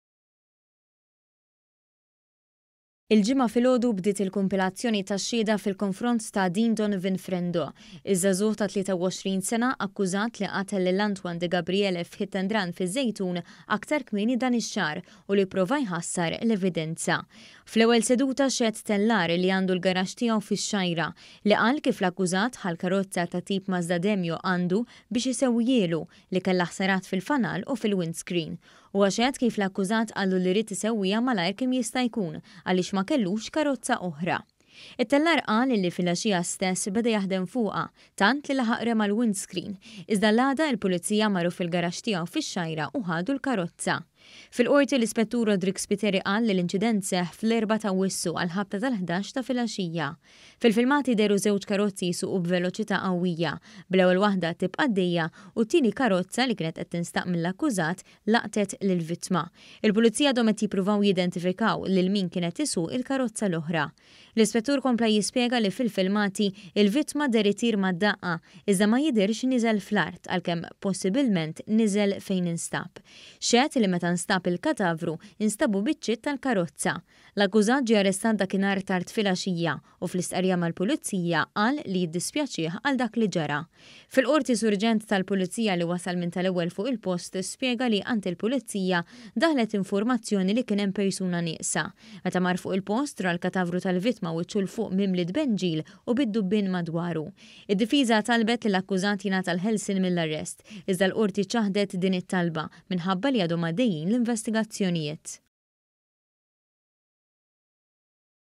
TIBDA L-KUMPILAZZJONI B’RABTA MAL-HIT AND RUN FIŻ-ŻEJTUN
ONE.COM.MT • Dalgħodu bdiet il-kumpilazzjoni tal-evidenza fil-konfront taż-żagħżugħ li jinsab akkużat b’hit and run fiż-Żejtun aktar kmieni dan ix-xahar, li ħalla lil Antoine Degabriele ta’ wieħed u ħamsin sena, mejjet.
Fost l-ewwel li xehdu kienu t-tellar li kellu l-karozza nvoluta fl-inċident u s-Surġent li sab lill-vittma mejjet.

FILMATI:  ARKIVJI

Il-Ġimgħa filgħodu bdiet il-kumpilazzjoni tal-evidenza fil-konfront ta’ Dean Donovan Frendo, iż-żagħżugħ ta’ tlieta u għoxrin sena akkużat li qatel lil Antoine Degabriele f’hit and run fiż-Żejtun aktar kmieni dan ix-xahar u pprova jħassar l-evidenza.

Fl-ewwel seduta xehed tellar li għandu l-garaxx tiegħu fix-Xgħajra, li qal kif l-akkużat ħa l-karozza tat-tip Mazda Demio għandu biex isewwihielu, li kellha ħsarat fil-fanal u fil-windscreen.

Huwa xehed kif l-akkużat qallu li ried isewwiha malajr kemm jista’ jkun għax ma kellux karozza oħra.

It-tellar qal li filgħaxija stess beda jaħdem fuqha tant li rema l-windscreen, u l-għada, il-Pulizija marru fil-garaxx tiegħu fix-Xgħajra u ħadu l-karozza.
Fil-Qorti, l-Ispettur Roderick Spiteri qal li l-inċident seħħ fl-erbgħa ta’ Awwissu għall-ħabta tal-ħdax ta’ filgħaxija.

Fil-filmati dehru żewġ karozzi jsuqu b’veloċità qawwija; bl-ewwel waħda tibqa’ għaddejja u t-tieni karozza, li kienet qed tinstaq mill-akkużat laqtet lill-vittma.

Il-Pulizija għadhom qed jippruvaw jidentifikaw lil min kien qed isuq il-karozza l-oħra.

L-Ispettur kompla jispjega li fil-filmati, il-vittma deher itir mad-daqqa iżda ma jidhirx nieżel fl-art, għalkemm possibilment niżel fejn instab.
Xehed li meta nstab il-katavru, instabu biċċiet tal-karozza.

L-akkużat ġie arrestat dakinhar tard filgħaxija u fl-istqarrija mal-Pulizija, qal li jiddispjaċih għal dak li ġara.

Fil-Qorti, Surġent tal-Pulizija li wasal minn tal-ewwel fuq il-post spjega li għand il-Pulizija daħlet informazzjoni li kien hemm persuna nieqsa.
Meta mar fuq il-post, ra l-katavru tal-vittma wiċċu ’l fuq mimli tbenġil u bid-dubbien miegħu.

Id-Difiża talbet li l-akkużat jingħata l-ħelsien mill-arrest, iżda l-Qorti ċaħdet din it-talba, minħabba li għadhom għaddejjin l-investigazzjonijiet.